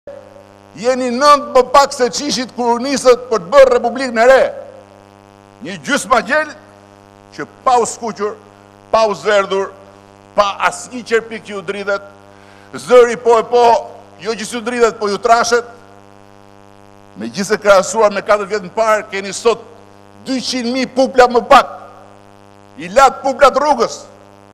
Ora News - Rama: PD nje gjysmagjel me 200 mijë pupla më pak. I late rruges
Sipas Rames PD eshte "gjysmagjel" që nuk ka reflektuar fare as nga rezultati i zgjedhjeve por nuk kanë ndryshuar as vullnetin e tyre destruktiv dhe negativ ndaj vendit.  Në këtë fushatë sipas Ramës është përfshirë dhe LSI e cila " dashurinë e qetësinë e ka zëvendësuar llapazanëri e vajmedetëri"

"Jeni 9 më pak se sa ishit kur shkuat për të bërë republikën e re në çadrën e dasmave. Gjysmagjel pa u skuqur e zverdhur dhe keni sot 200 mijë pupla më pak. I latë puplat rrugës. Bëni trimin dhe ngrini volumin njësoj si frikacaku që këndon kur merr pyllin.

Shto këtu edhe miqtë e LSI, që qetësinë dhe dashurinë e linjës së vjetër e zëvendësuan me llafazanërinë dhe vajmdedetërinë e linjës së re dhe panorama e opozitës bëhet:

“Bir o Lul vëllai kë të qaj më parë, ty a motrën tonë që keni një hall”.

Po kjo natyrisht është një çështje e kotë, retorike sepse ka më shumë emocion se kë të qash më parë, vëllain apo motrën që kanë një hall dhe që halli i ka bërë bashkë si një kazan që ky nga ku dëgjohen thirrjet bombastike si të të luftojmë krimin, drogën se në fakt krimi dhe droga e kanë fajin që ju jeni papërgjegjshmëri.